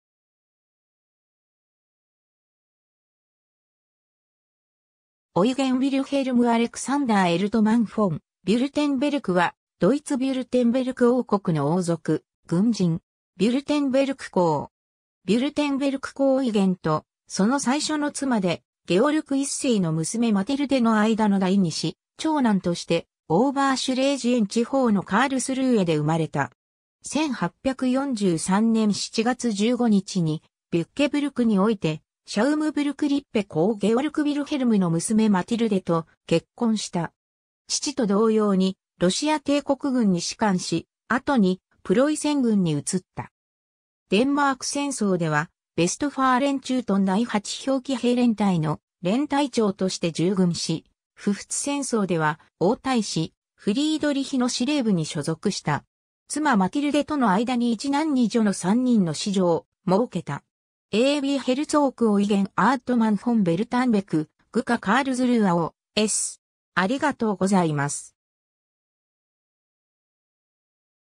オイゲン・フォン・ヴュルテンベルク (1820-1875)
ヴュルテンベルク＝カールスルーエ家
オイゲン・ヴィルヘルム・アレクサンダー・エルトマン・フォン・ヴュルテンベルク（Eugen Wilhelm Alexander Erdmann Herzog von Württemberg, 1820年12月25日 - 1875年1月8日）は、ドイツ・ヴュルテンベルク王国の王族、軍人。
ヴュルテンベルク公（Herzog von Württemberg）。
 ヴュルテンベルク公オイゲンと、その最初の妻でゲオルク1世の娘マティルデ（1801年 - 1825年）の間の第2子、長男として、オーバーシュレージエン地方のカールスルーエ（現在のポーランド領オポーレ県ナムィスウフ郡）で生まれた。
1843年7月15日にビュッケブルクにおいて、シャウムブルク＝リッペ侯ゲオルク・ヴィルヘルムの娘マティルデ（1819年 - 1891年）と結婚した。
父と同様にロシア帝国軍に仕官し、後にプロイセン軍に移った。
デンマーク戦争ではヴェストファーレン駐屯第8驃騎兵連隊（Husaren-Regiment „Kaiser Nikolaus II. von Russland“ (1. Westfälisches) Nr. 8）の連隊長として従軍し、普仏戦争では王太子フリードリヒの司令部に所属した。 妻マティルデとの間に1男2女の3人の子女をもうけた。 ^ a b Herzog Eugen Erdmann von Württemberg(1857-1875) - GKA Carlsruhe O/S